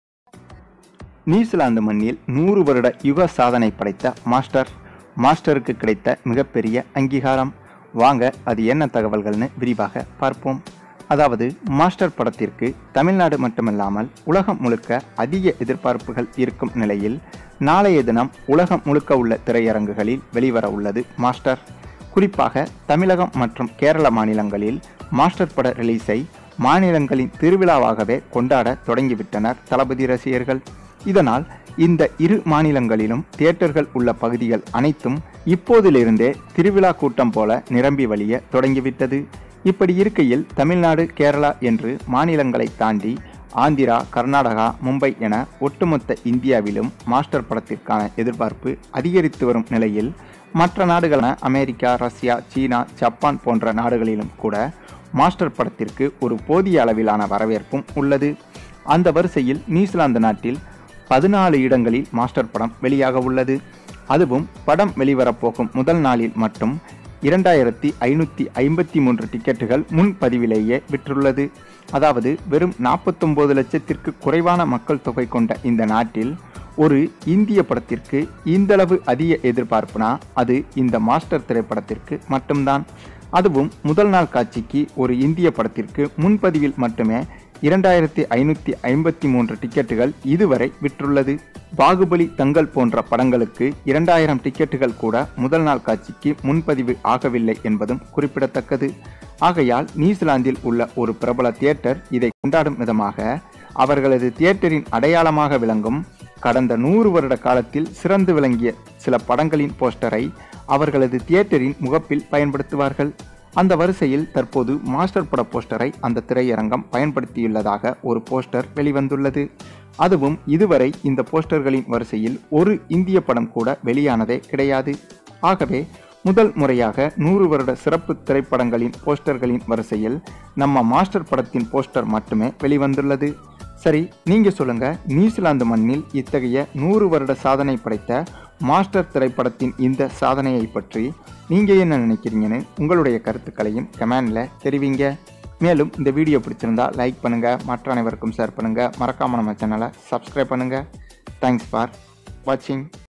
நியூசிலாந்து மண்ணில் மாஸ்டர் 100 வருட "யுக" சாதனை மரண சம்பவம் செய்த தளபதி
Master Pongal Release | Master Newzealand Biggest Release | Newzealand Master Release Record | Master World Record | Vijay movie World Release |